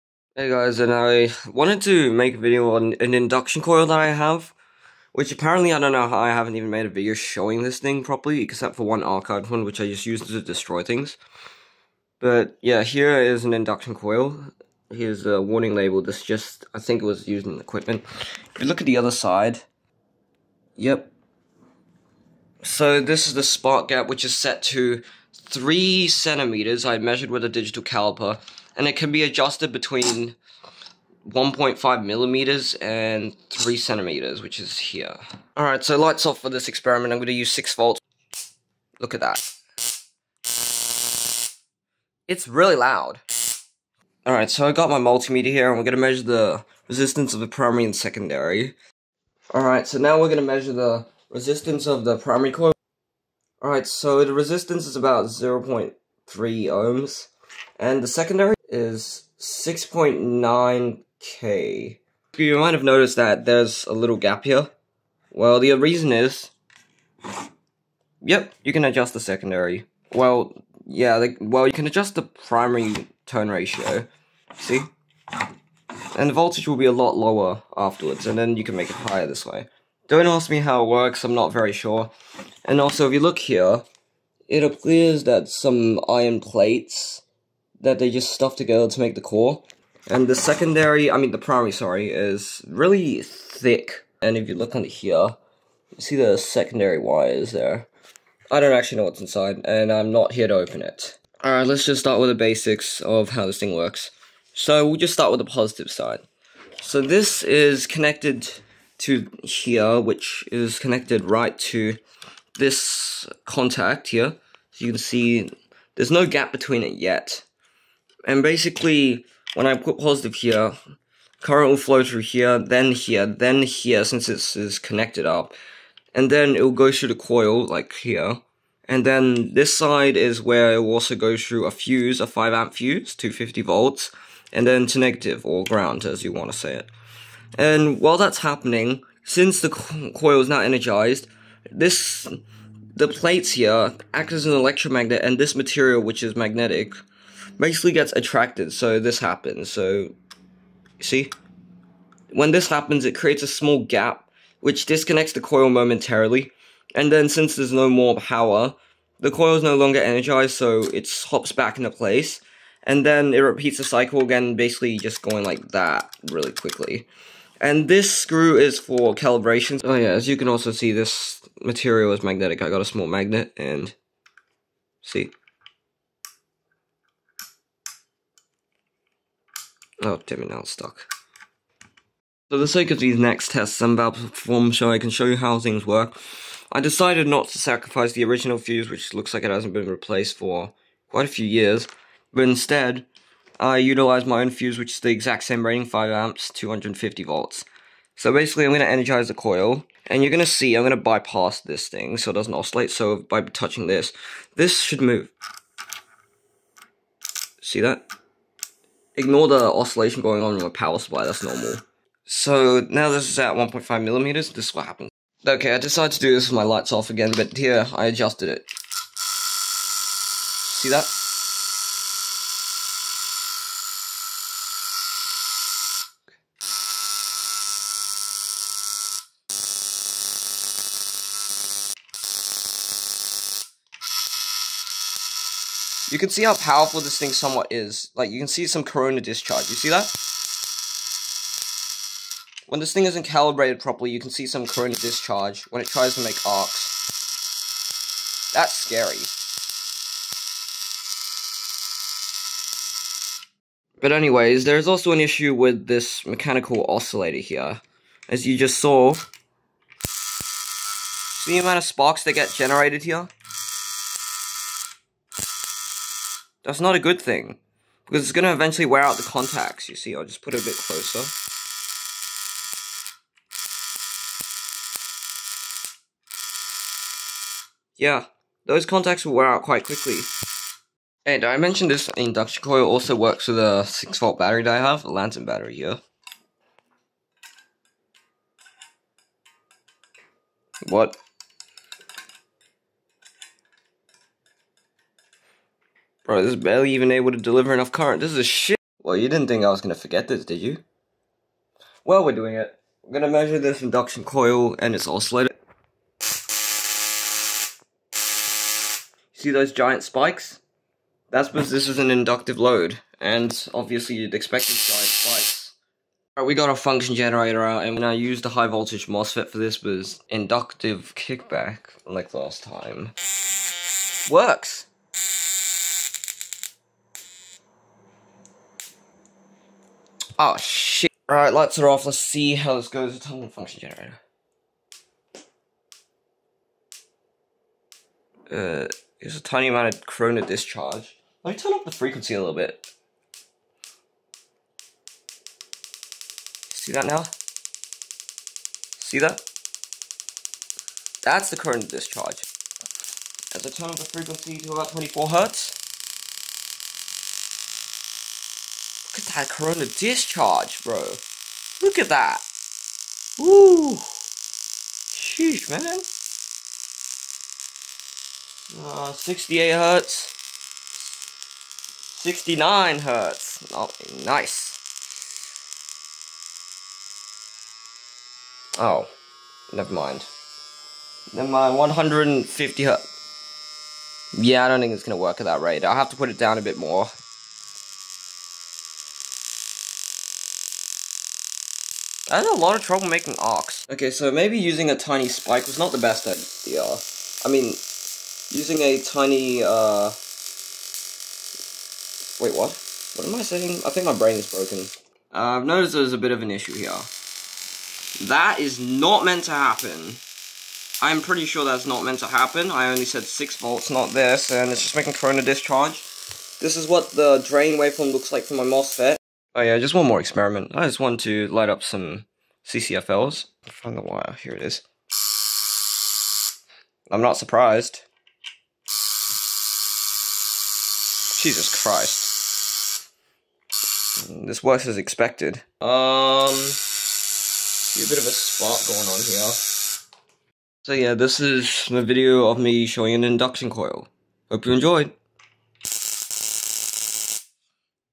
Induction Coil
Just showing an old induction coil that my dad had for many years now. I am thinking the induction coil didn't work as well with the MOSFET since the RDS of the W14NK50Z was quite high being 380mΩ (this is normal with high voltage MOSFETs). I only used the W14NK50Z to avoid the MOSFET being killed by the high voltage spikes.

0:00 Introduction
0:36 High Voltage Arcs
0:44 Measuring Transformer Resistance
1:03 More Explaining
1:42 Functionality
3:03 Fuses
3:19 Demonstration
3:32 High Voltage Arcs
3:54 Corona Discharge
4:14 More Explaining
4:41 6V Battery Fail
4:59 Waveforms - Mechanical Oscillator
5:17 Solid State Driver (MOSFET + Function Generator)
5:24 Inductive Kickback Flashback lol
5:32 High Voltage Arcs (More like corona discharge) (MOSFET Driven)
6:34 Waveforms - MOSFET Drain
6:46 Power Supply??
6:56 Waveforms - MOSFET Drain
7:00 CCFL Test
7:21 Conclusion